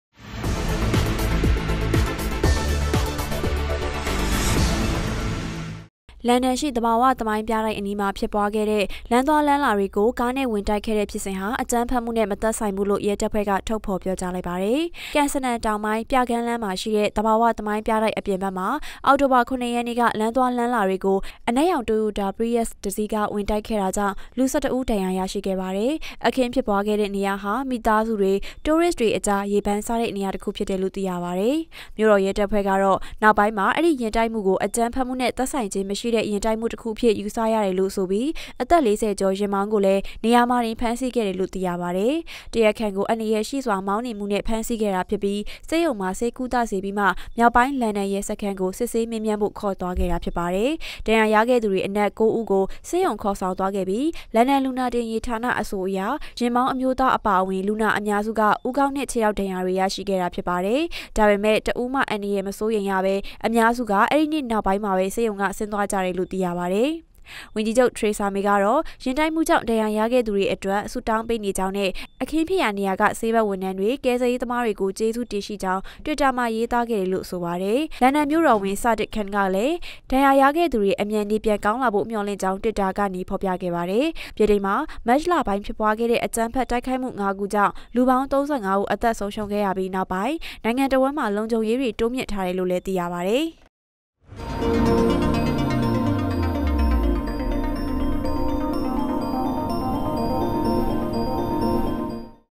လန္ဒန္တြင္လမ္းသြားလမ္းလာမ်ားအားကားျဖင္႔၀င္တိုက္ခဲ႔သည္႔ျဖစ္စဥ္သည္အၾကမ္းဖက္မႈႏွင္႔
လန္ဒန္တြင္လမ္းသြားလမ္းလာမ်ားအားကားျဖင္႔၀င္တိုက္ခဲ႔သည္႔ျဖစ္စဥ္သည္အၾကမ္းဖက္မႈႏွင္႔ မသက္ဆိုင္ဟု ရဲတပ္ဖြဲ႕ေျပာ
လန္ဒန္ရွိသဘာ၀သမိုင္းျပတိုက္အနီးမွာျဖစ္ပြားခဲ့တဲ့ လမ္းသြားလမ္းလာေတြကို ကားနဲ႔၀င္တိုက္ခဲ႔တဲ႔ျဖစ္စဥ္ဟာ အၾကမ္းဖက္မႈနဲ႔ မသက္ဆိုင္ဘူးလို႔ရဲတပ္ဖြဲ႕က ထုတ္ေဖာ္ေျပာၾကားလိုက္ပါတယ